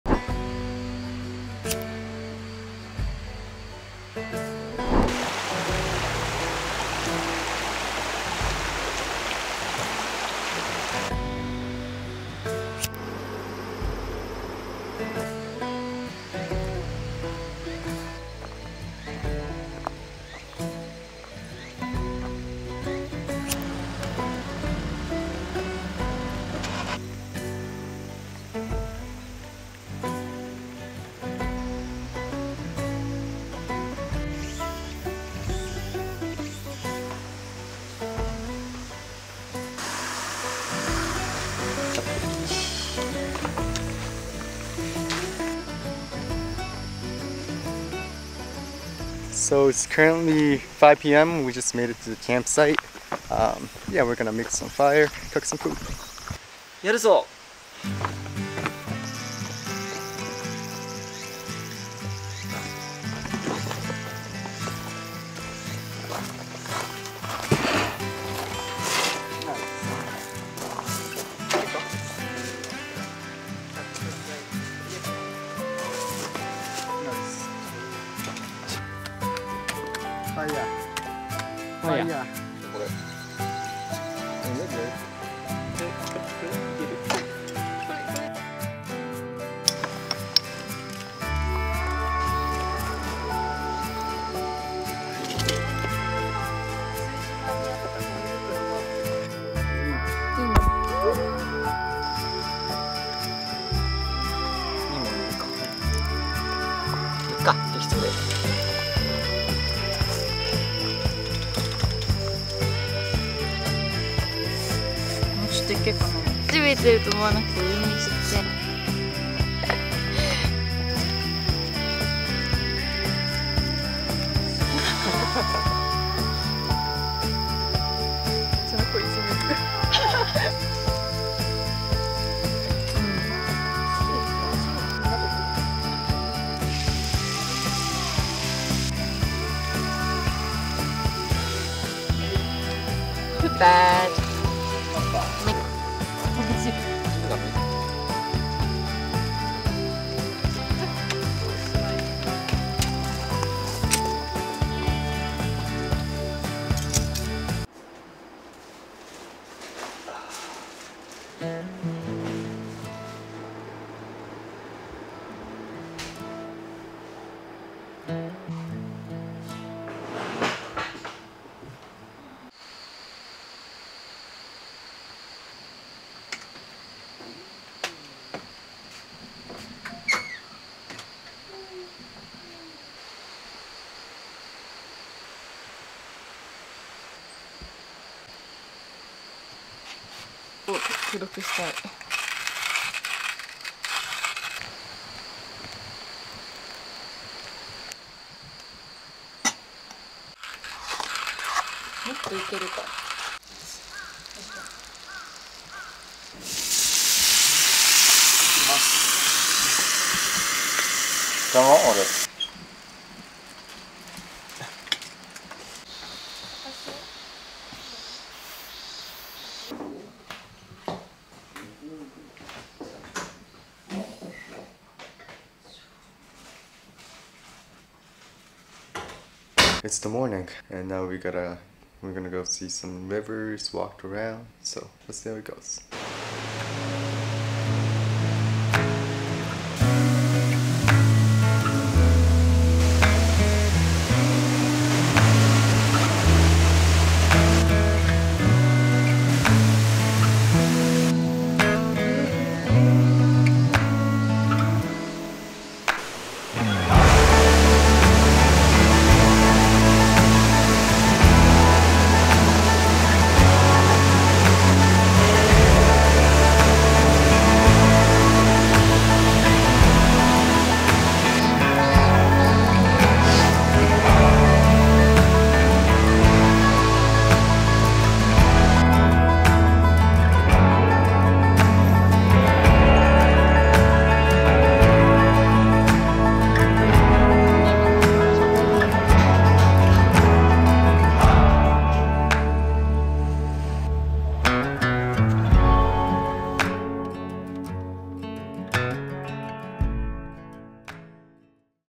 Camping in Japan
A weekend camping in Shizuoka to heal my mind and soul.

Hello there! I'm Kai.

(maybe I can help you or include it in the next video)